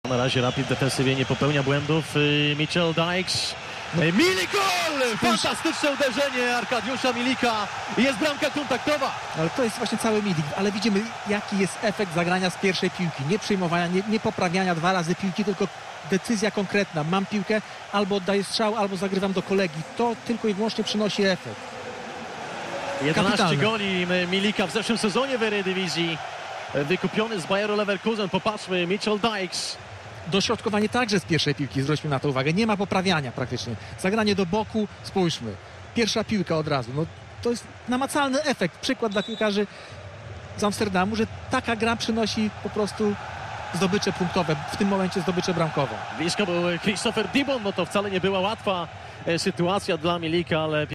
Arkadiusz Milik Amazing Volley Goal | Ajax Amsterdam 1 - 2 SK Rapid Wien | UCL 2015 HD
What a hit! Arkadiusz Milik (Ajax) shows good composure inside the box to fire a first-time shot into the bottom right corner. 1:2.